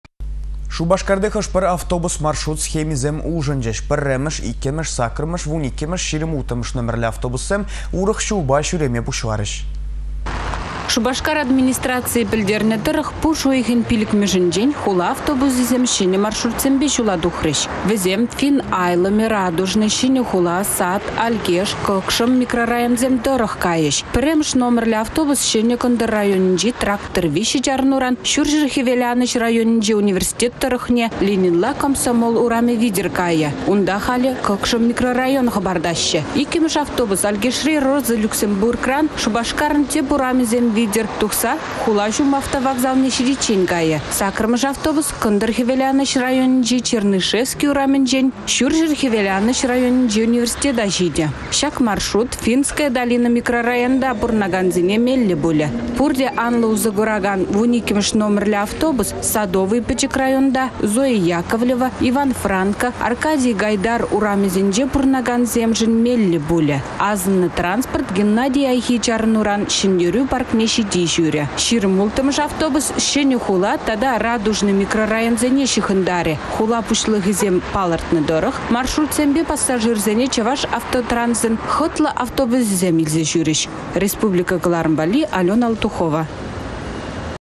Хула автобусĕсем - çĕнĕ маршрутпа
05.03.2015 Шупашкарти хăш-пĕр автобус маршрут схемисем улшăнчĕç. 1-мĕш,2-мĕш,8-мĕш,12-мĕш,26-мĕш номерлĕ автобуссем урăх çулпа çÿреме пуçаларĕç